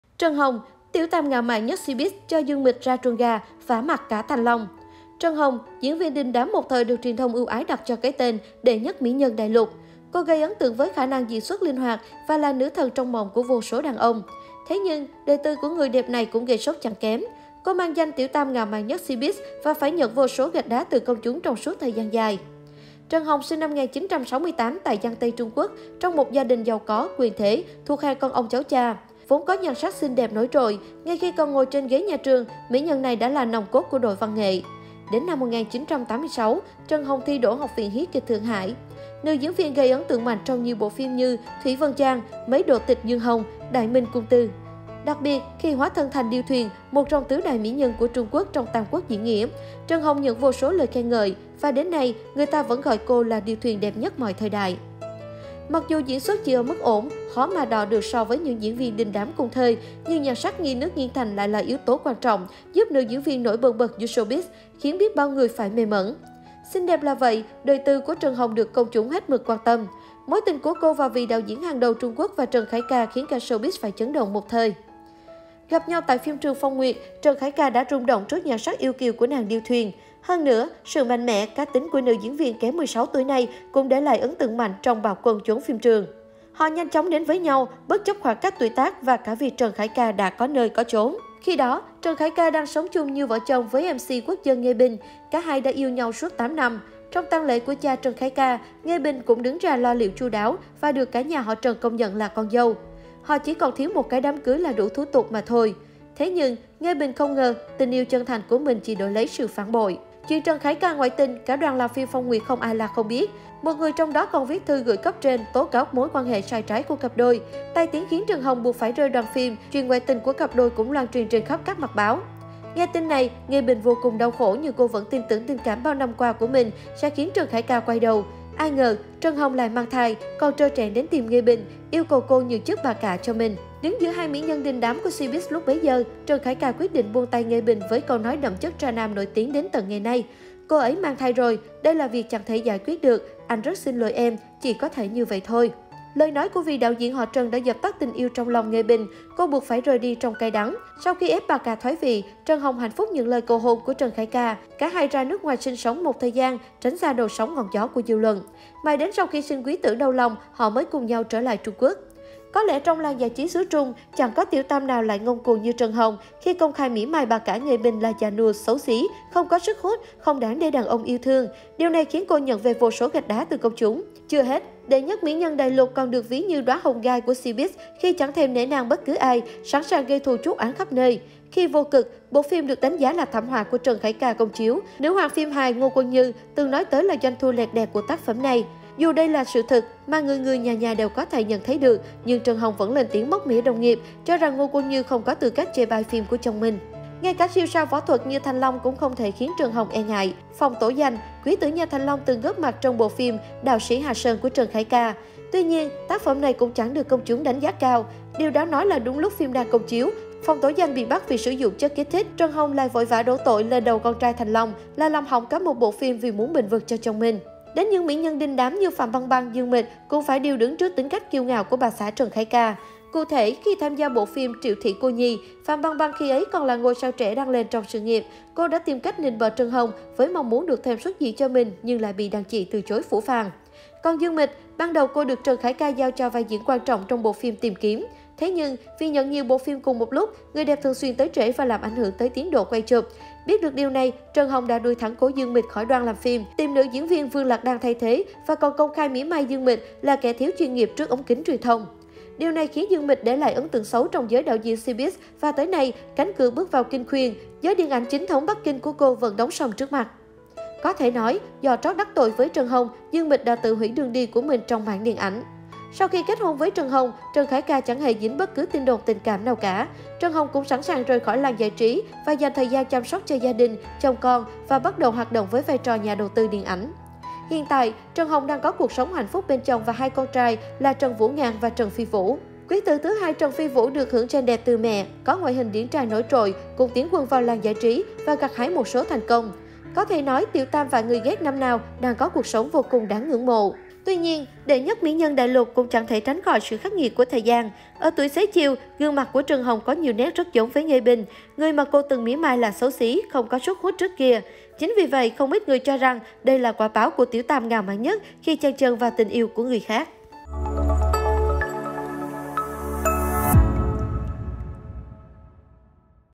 Trần Hồng - Tiểu tam ngạo mạn nhất Cbiz cho Dương Mịch “ra chuồng gà”, “vả mặt” cả Thành Long
Trần Hồng - diễn viên đình đám một thời được truyền thông ưu ái đặt cho cái tên “Đệ nhất mỹ nhân Đại lục". Cô gây ấn tượng với khả năng diễn xuất linh hoạt và là nữ thần trong mộng của vô số đàn ông. Thế nhưng, đời tư của người đẹp này cũng gây sốc chẳng kém. Cô mang danh "Tiểu tam ngạo mạn nhất Cbiz" và phải nhận vô số "gạch đá" từ công chúng trong suốt thời gian dài.